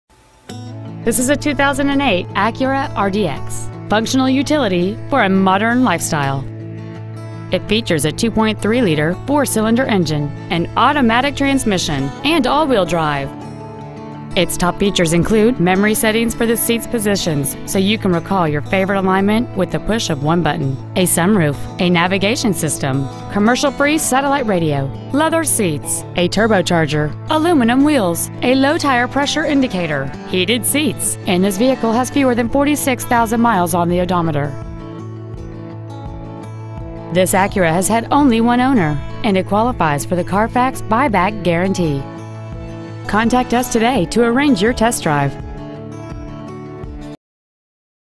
Pre-Owned 2008 Acura RDX Lawrenceville NJ
We are proud to present this 2008 Acura RDX.

 We've been honored to serve the Lawrenceville NJ area, we promise that your experience at our dealership will exceed your expectations!

 Year: 2008
 Make: Acura
 Model: RDX
 Engine: 4 Cyl - 2.3L
 Trans.: Automatic
 Exterior: Polished Metal Metallic
 Miles: 45,400